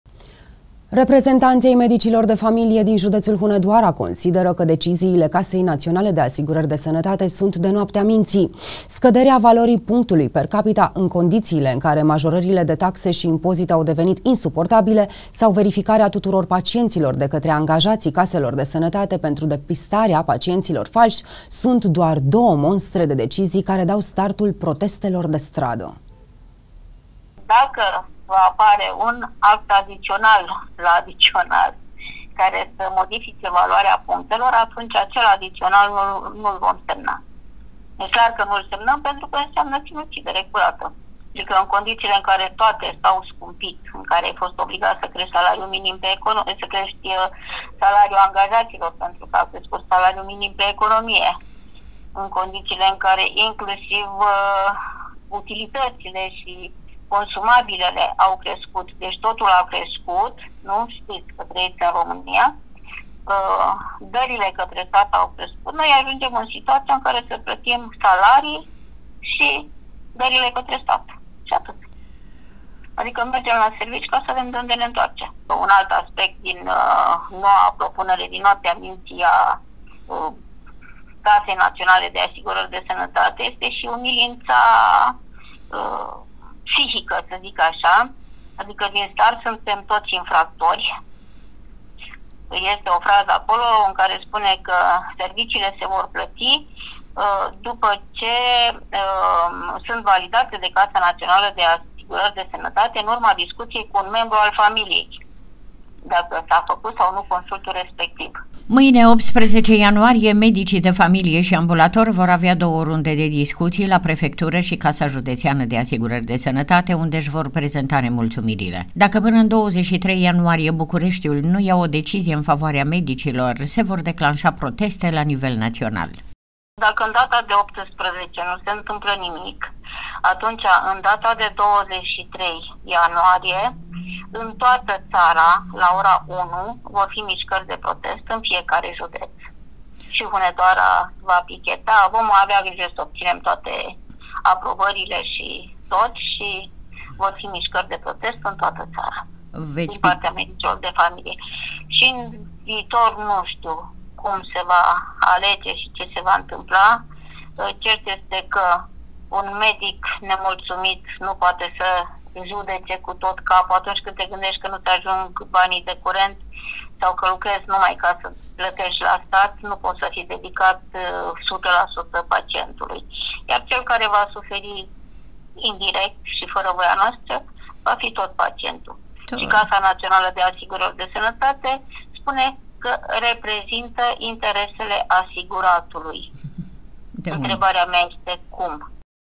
MEDIC: „C.N.A.S. NE TRATEAZĂ CA PE NIȘTE INFRACTORI”
PROTESTE ALE MEDICILOR DE FAMILIE ÎN TOATĂ ȚARA MARȚI, 23 IANUARIE
        Reprezentanți ai medicilor de familie din județul Hunedoara consideră că deciziile casei naționale de asigurări de Sănătate sunt de „noaptea minții”. Scăderea valorii punctului per capita, în condițiile în care majorările de taxe și impozite au devenit insuportabile, sau verificarea tuturor pacienților de către angajații caselor de sănătate pentru depistarea pacienților falși, sunt doar două mostre de decizii care dau startul protestelor de stradă.